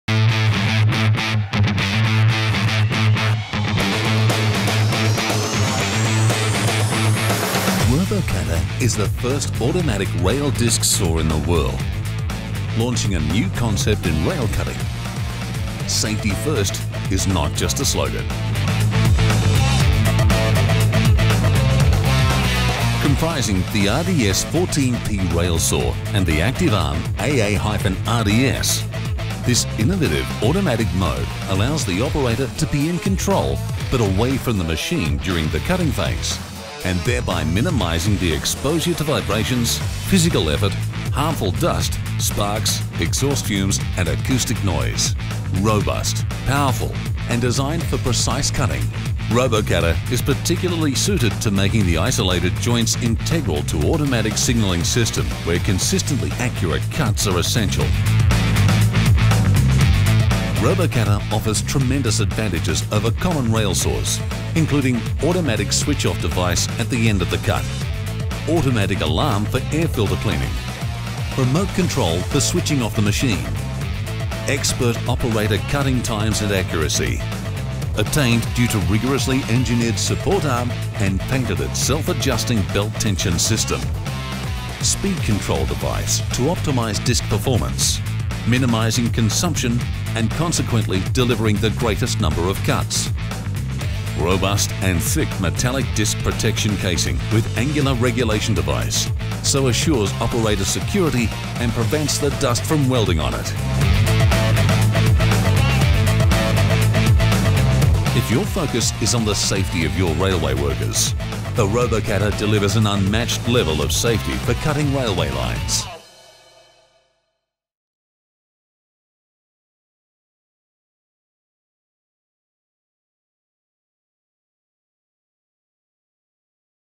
CEMBRE - Automatic rail disc saw ROBOKATTA
Comprising the RDS-14P railsaw and the “active” arm AA-RDS, Robokatta is a new concept in Rail Disc Saws.
Robust, powerful and designed for precise cutting and with a perfectly perpendicular cutting action, Robokatta is particularly suited to making the isolated joints integral to automatic signalling systems, where consistently accurate right-angle cuts in the horizontal and vertical axes are essential.

The innovative automatic mode allows the operator to be in control, but away from the machine during the cutting phase, and thereby exposed to:

Vibration: Zero
Physical Effort: Zero
Harmful Dust: Zero
Sparks: Zero
Exhaust Fumes: Zero
Acoustic Noise: Minimum